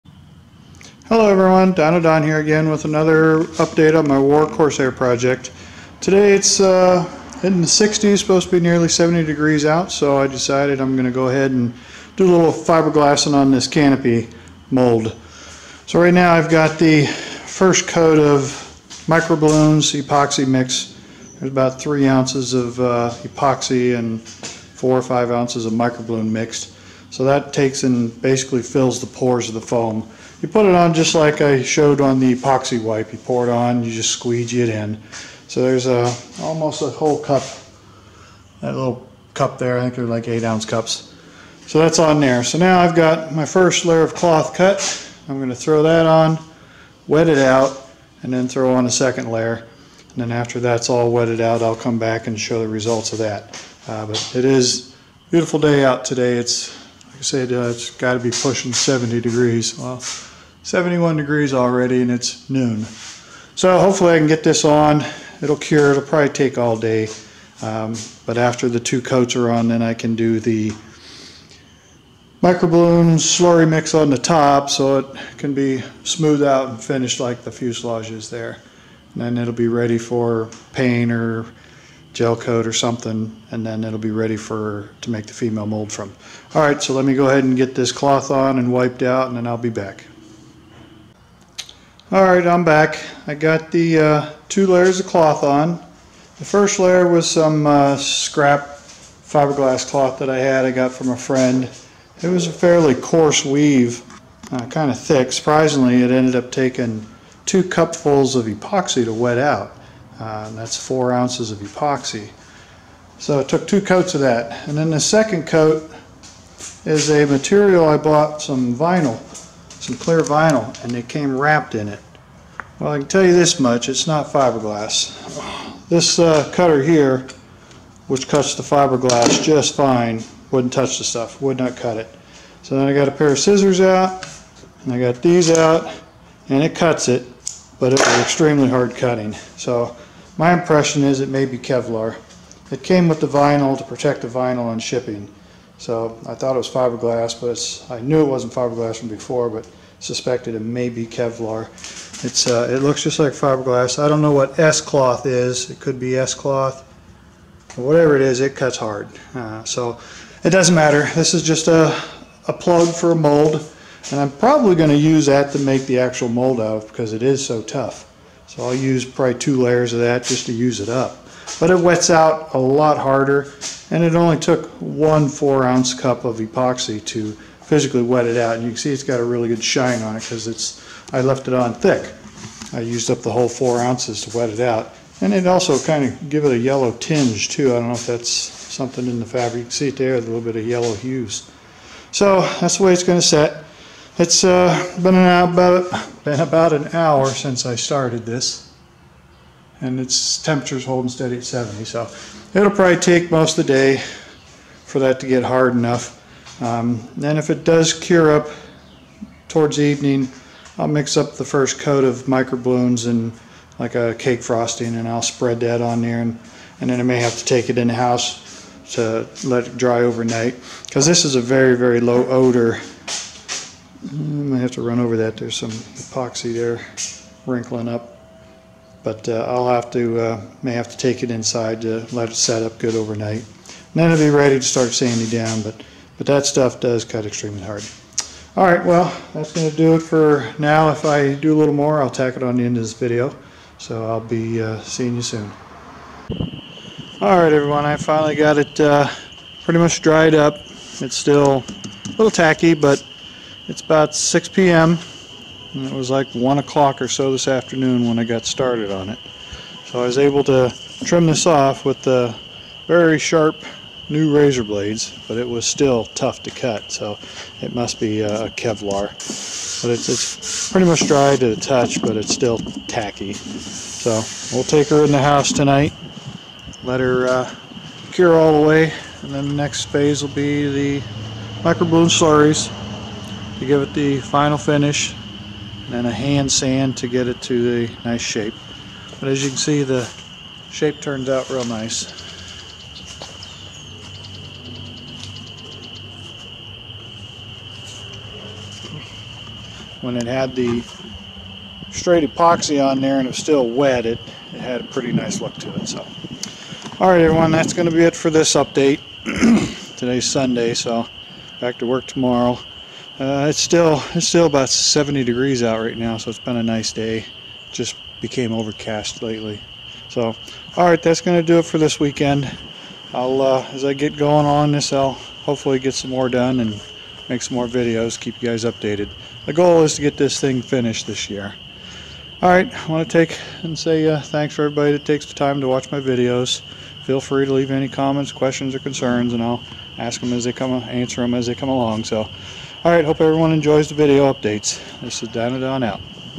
W.A.R. Corsair update 4/7/2019
Today I put two layers of cloth over the canopy plug. This is just a male plug from which a female mold will be made from. Then the female mold will be used to vacuum form the plexiglass canopy. Next step will be the final filling and sanding, to get it ready for paint or gelcoat.